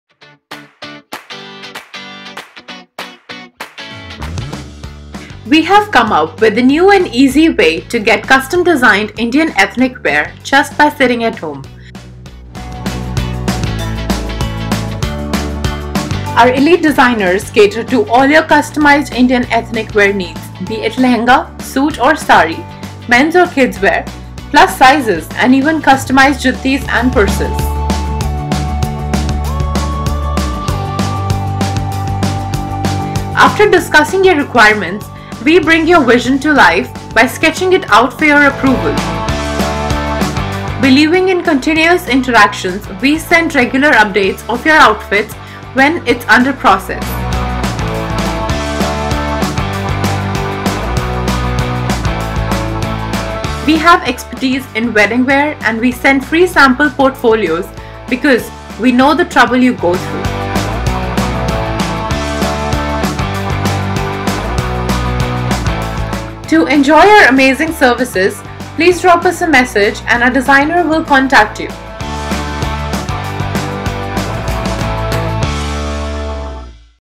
How we make custom designed Indian ethnic outfits | GetEthnic.com
Explaining how we make awesome custom designed outfits for you!!